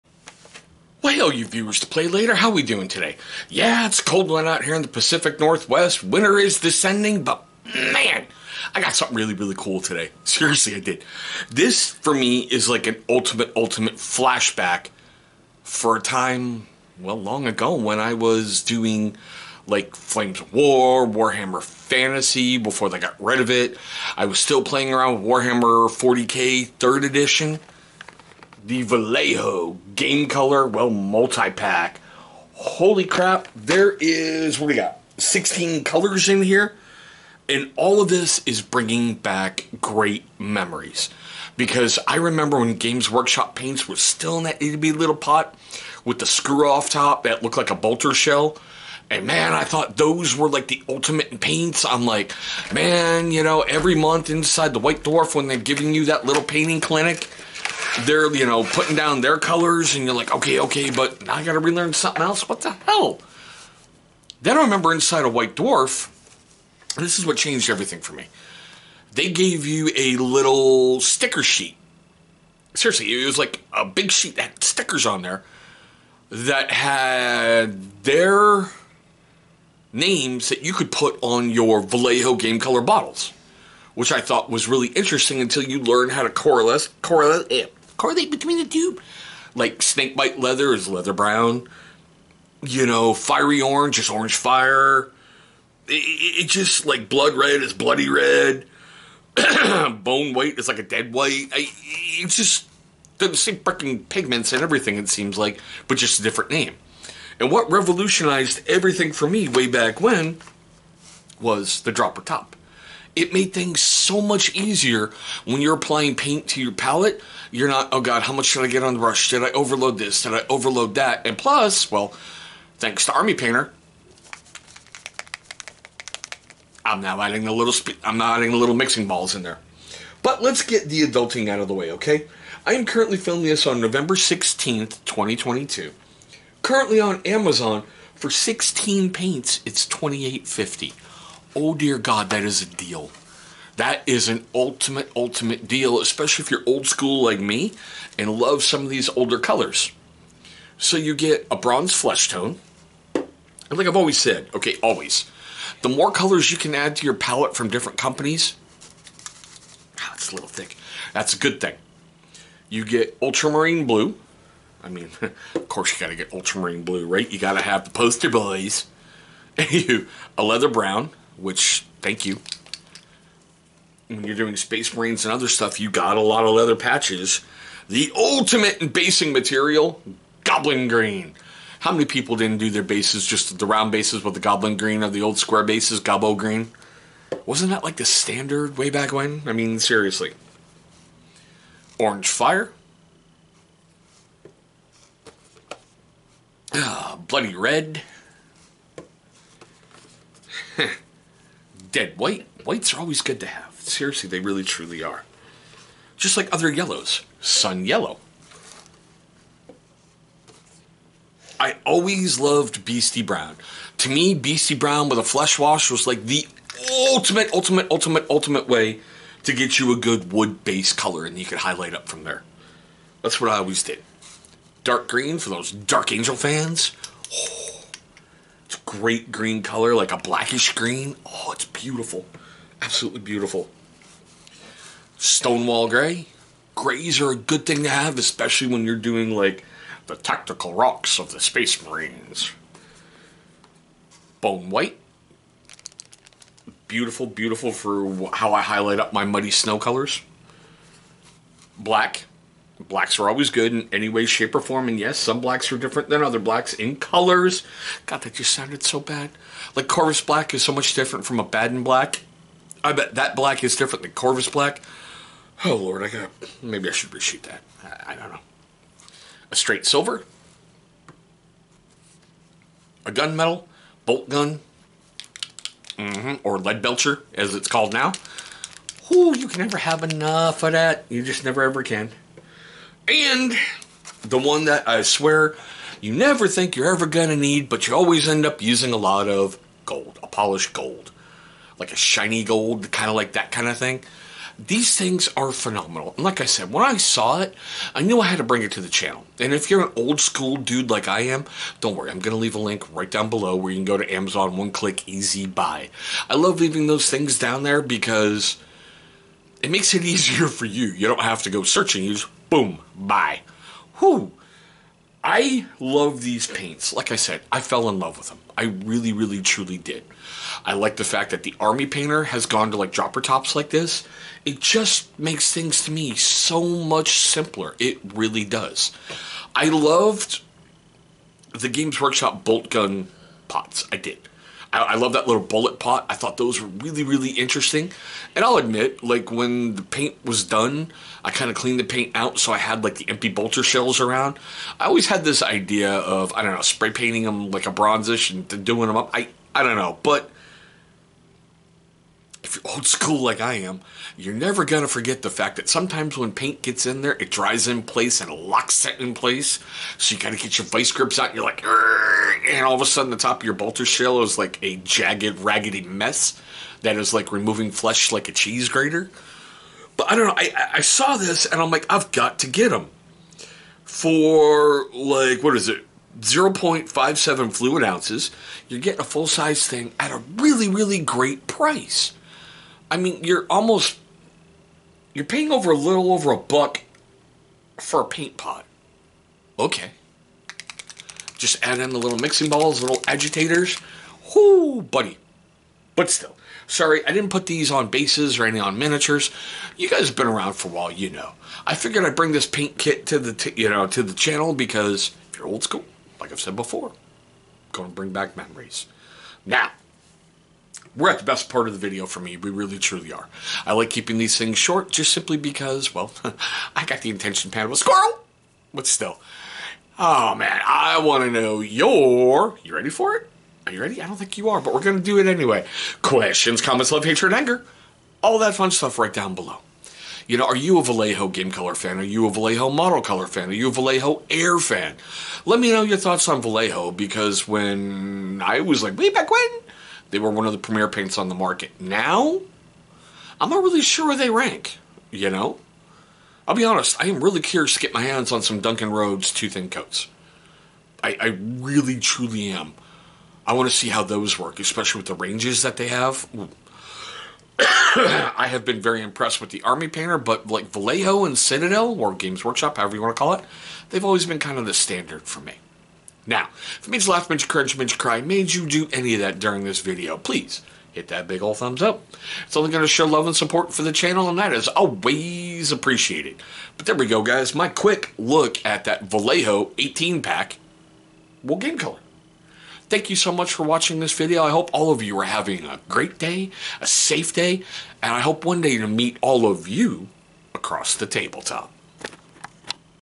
Vallejo Game Color 16 Piece Paint Set
Paints, an evil need of our hobby. There are lots to choose from. Today on the channel we are going to look at Vallejo Game Color 16 Piece Paint Set. Vallejo game color was my go to paints for a very VERY long time.

If you would like to add these paints to your collection, I have left a link to amazon.com